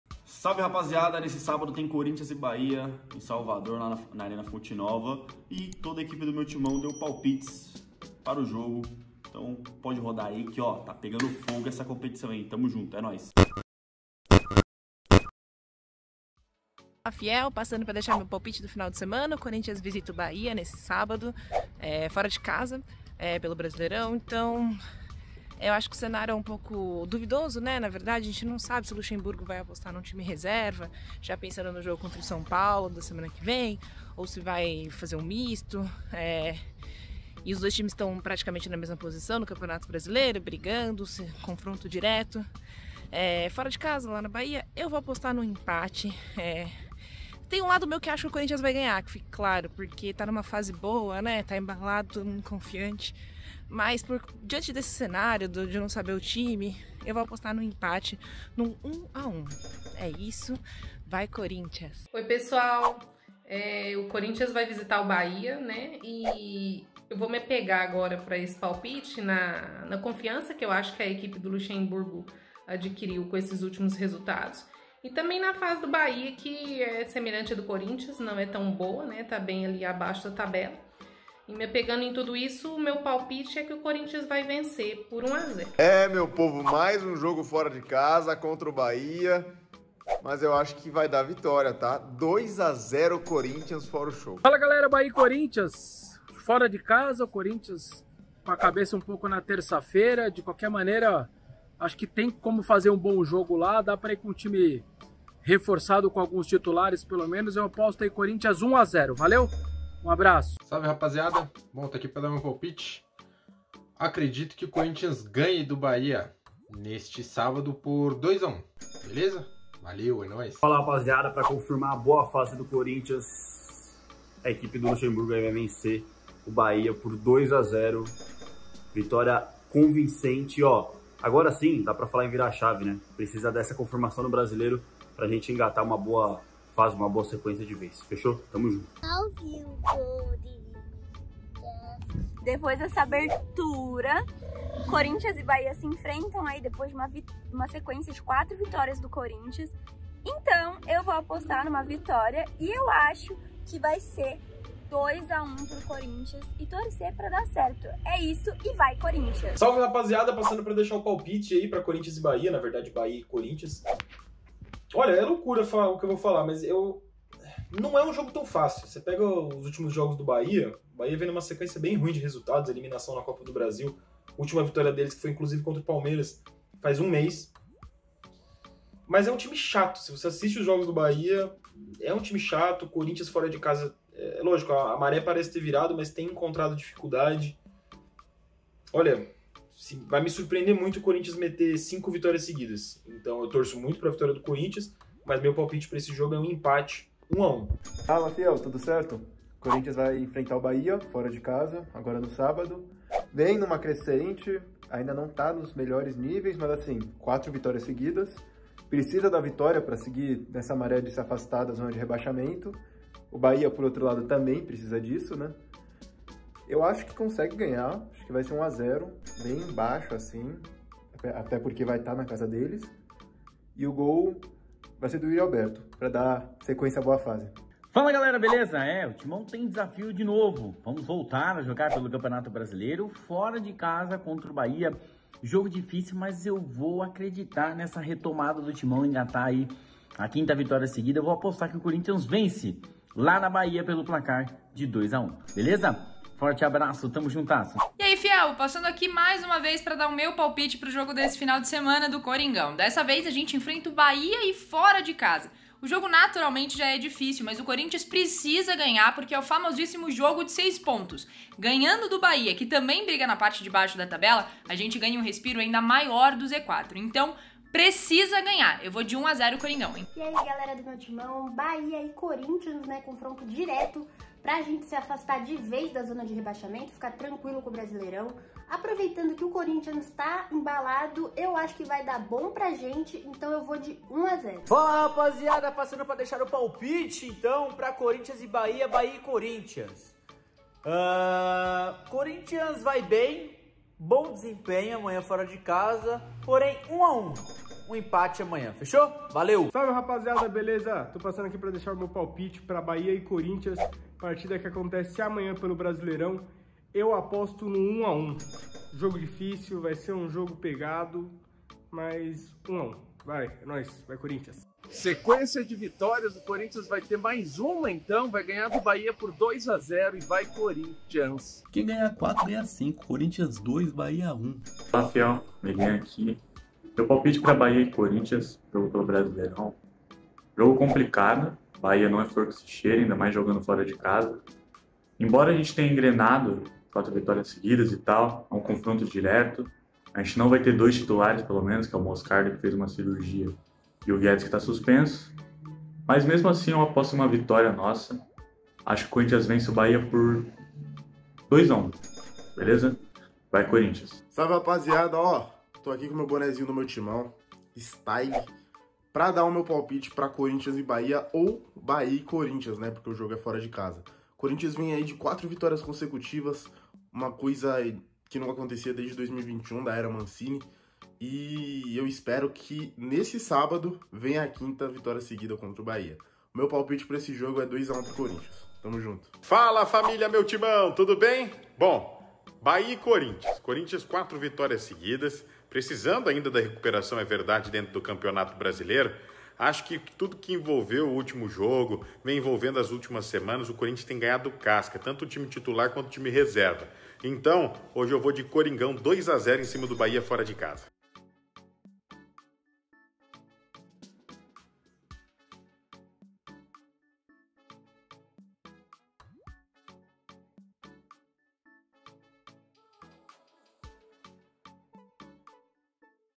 BAHIA x CORINTHIANS | Palpites Meu Timão | Brasileirão 2023
O Corinthians vai até Salvador para enfrentar o Bahia, pela 16° rodada do Campeonato Brasileiro 2023 e, no clima pré-jogo, você deixa seus palpites NOS COMENTÁRIOS aqui do vídeo!

Antes, claro, veja como está o otimismo (ou não) da equipe do Meu Timão com nossos pitacos para o jogo deste sábado.